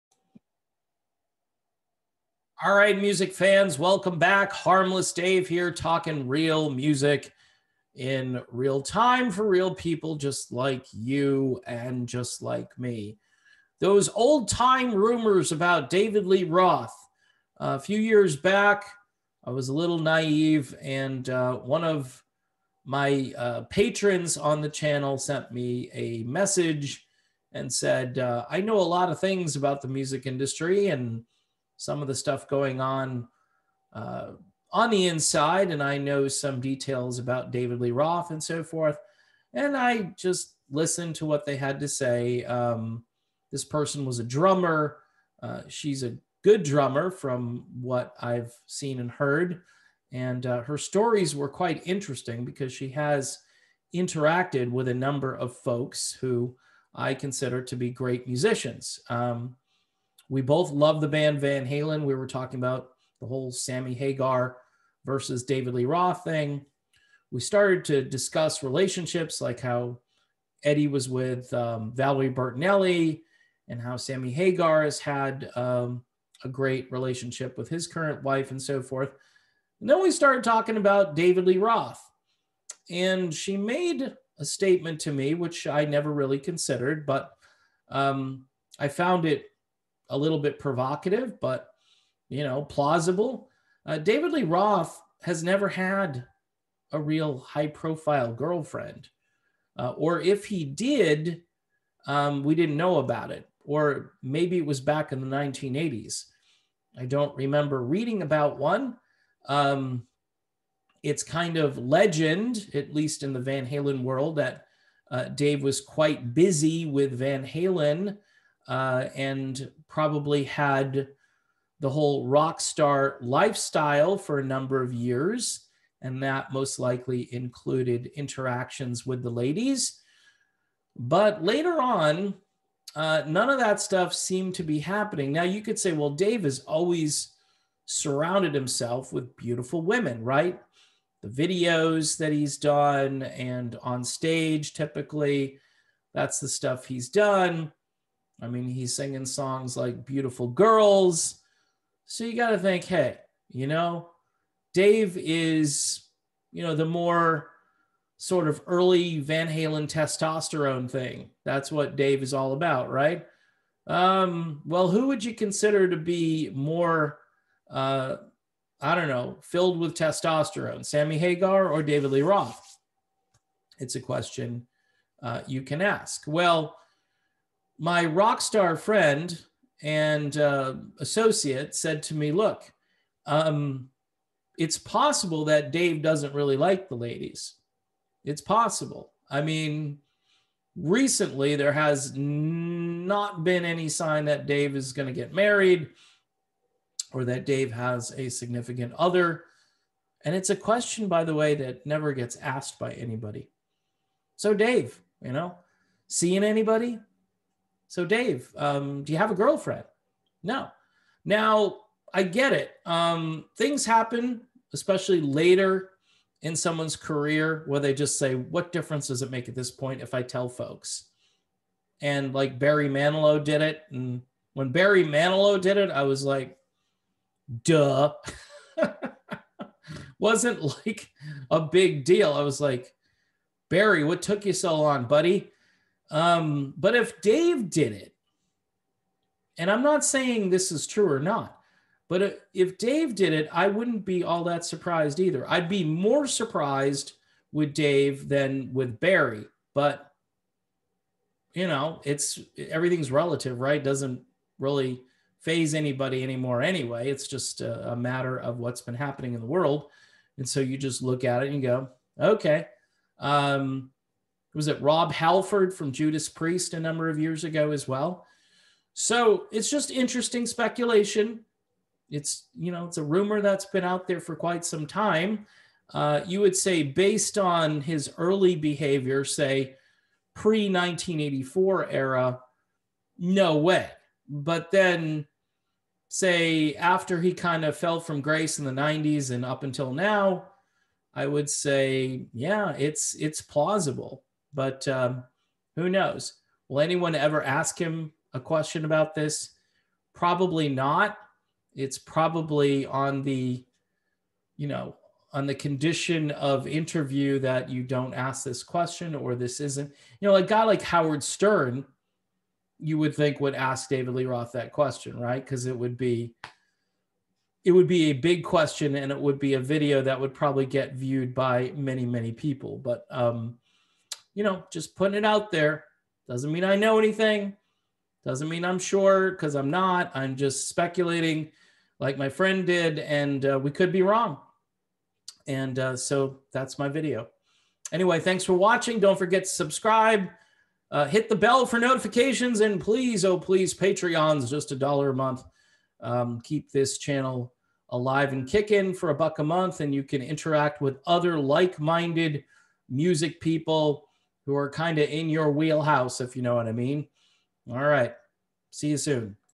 Those Rumors About David Lee Roth
This video is purely speculative. I do not have any solid evidence of what is put forward here. This is a think piece and nothing more.